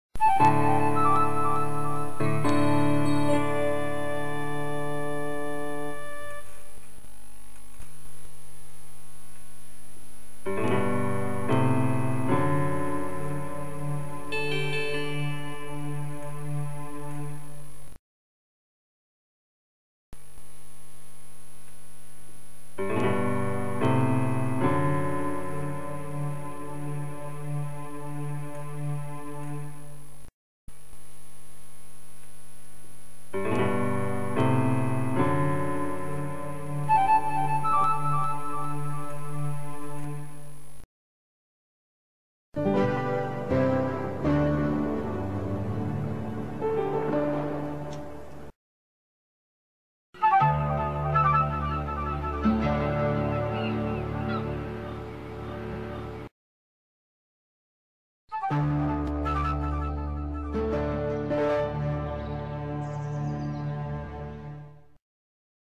Westcountry Mocks 2
A second video worth of Westcountry Ident mocks, based on the idents shown on Westcountry in the 90s. I took the photographs for the idents while I was on Holiday last year and this year